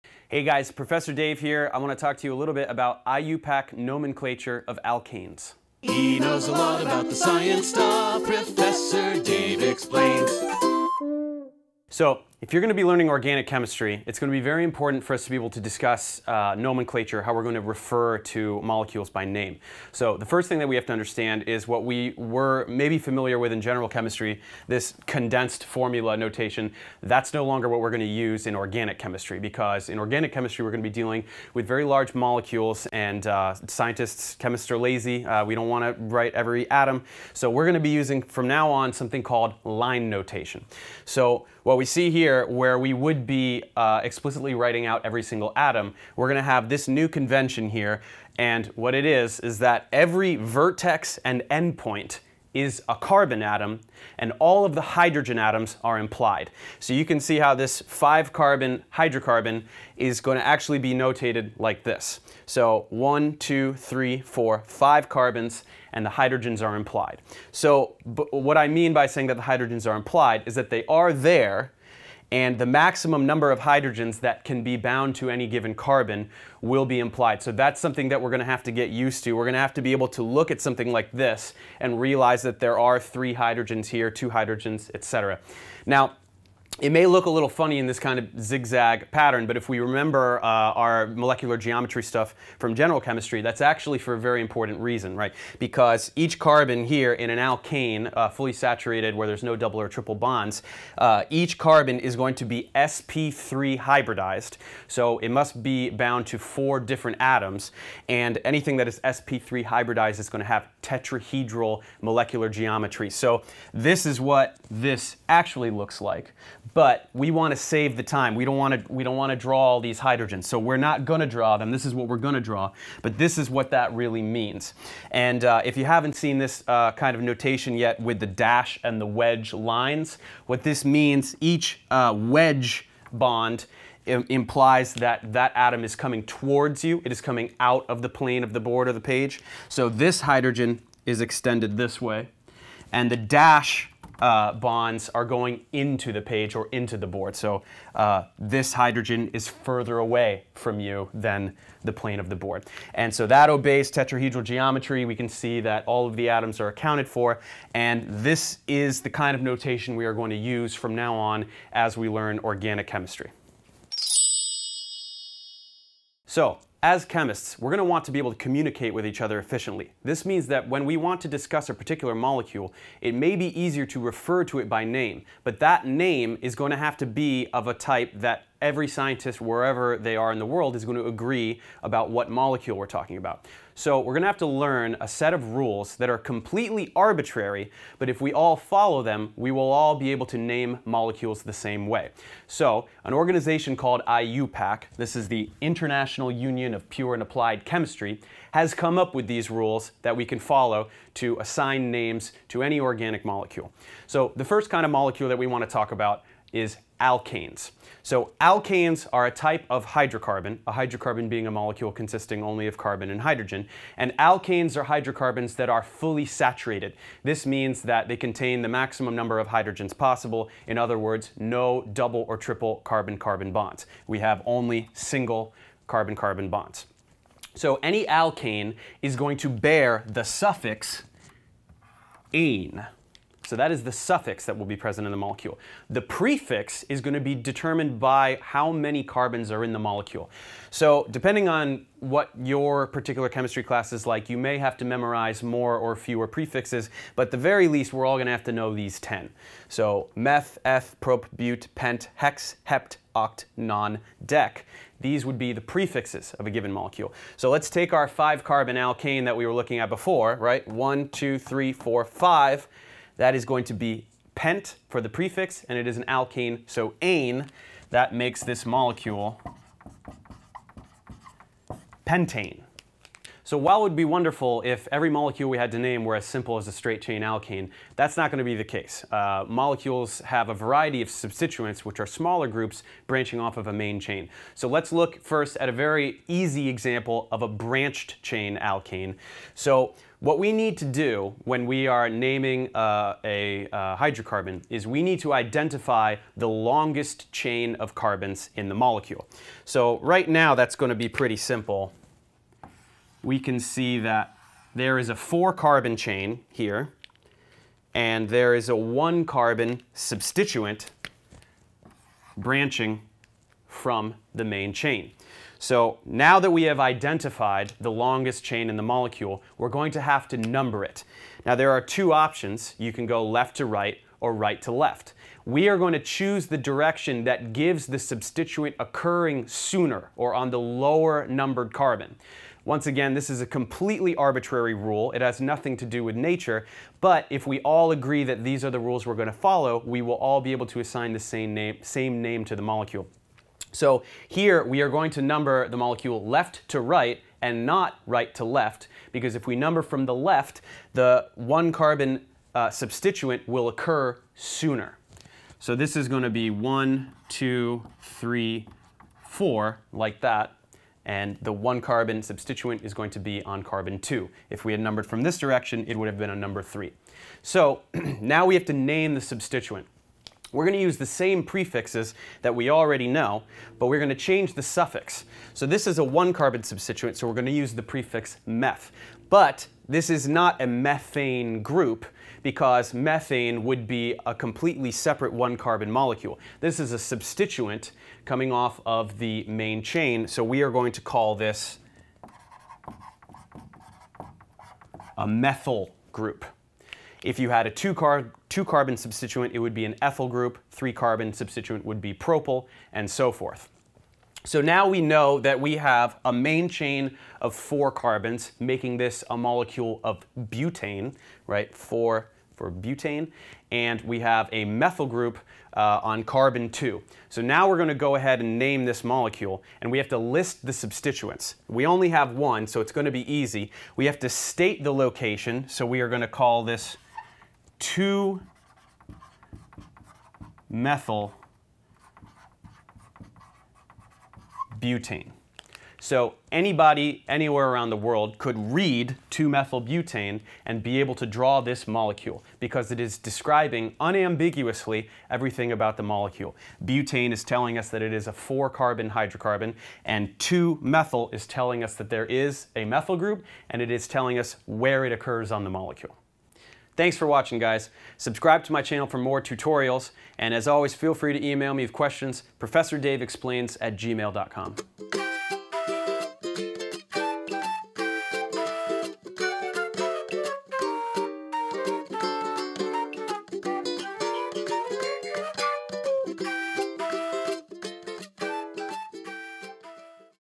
IUPAC Nomenclature of Alkanes: Part 1
Are you confused by IUPAC nomenclature? Let Professor Dave help! First I'll explain what line notation represents, and then I'll introduce the basic rules for naming alkanes.